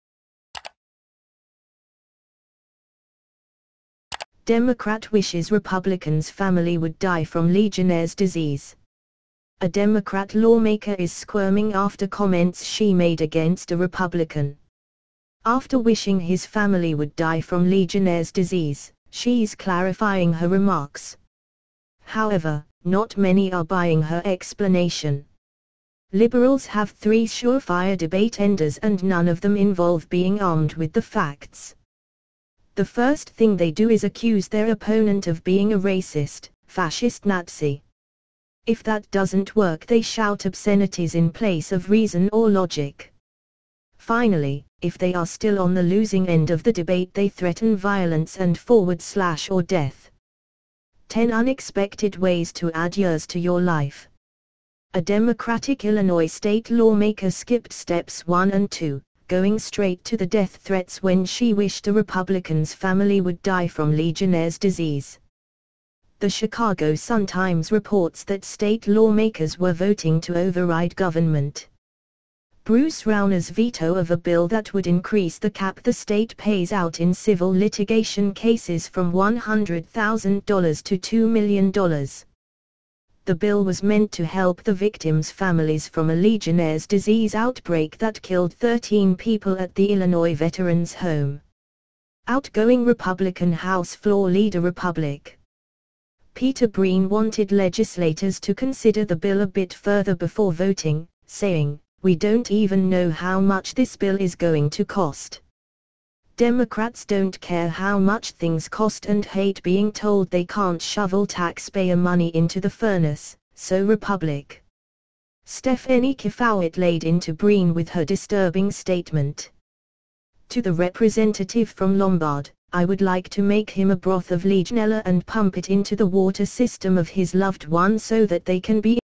Democrat Wishes Republican’s Family Would Die From Legionnaires’ Disease Breaking news
Breaking news Democrat Wishes Republican’s Family Would Die From Legionnaires’ Disease